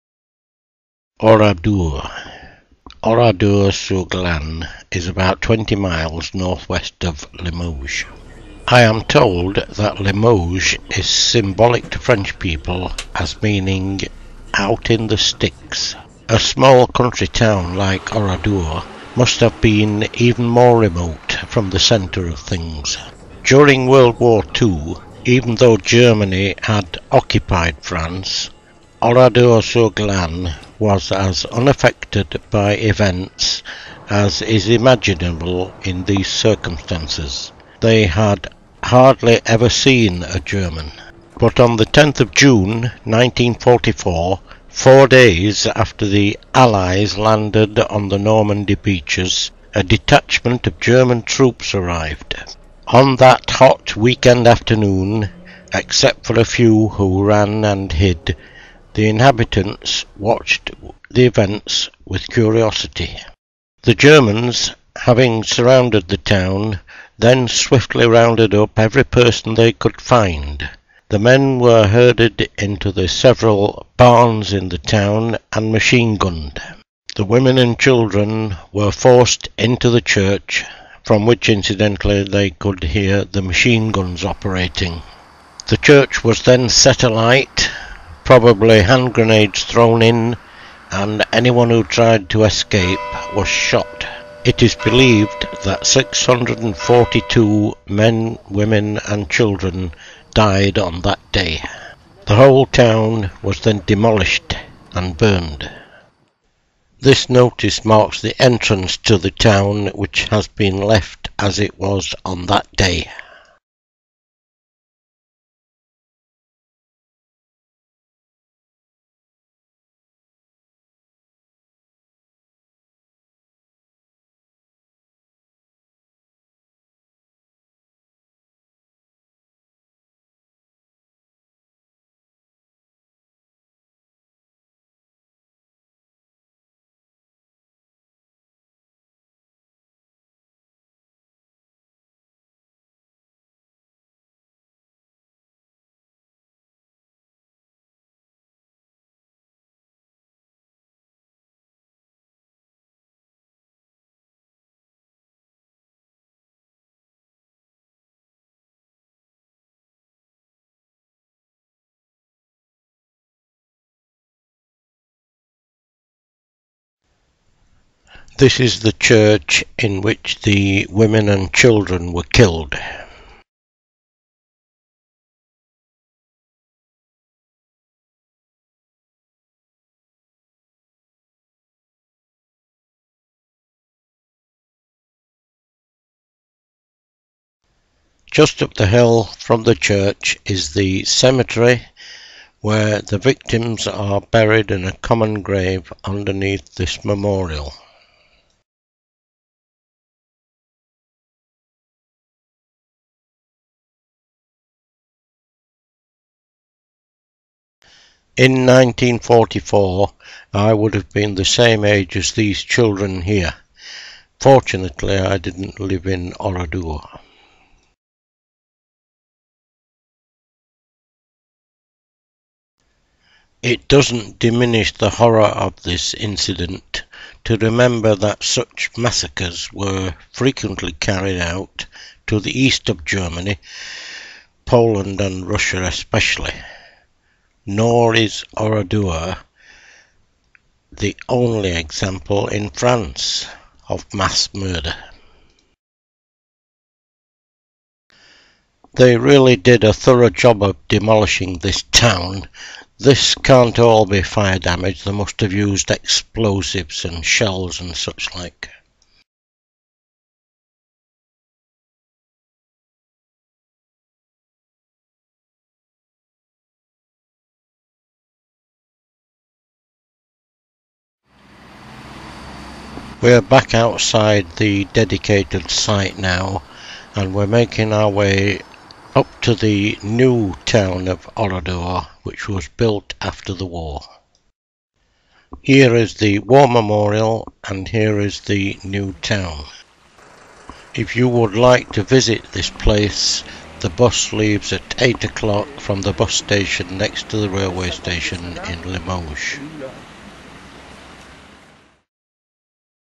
Peter Marshall's France 7 Part 2 Oradour
Take a local bus to Oradour-sur-Glane.  The town was completely destroyed on 10 June 1944  after its 600+ inhabitants had been rounded up and killed.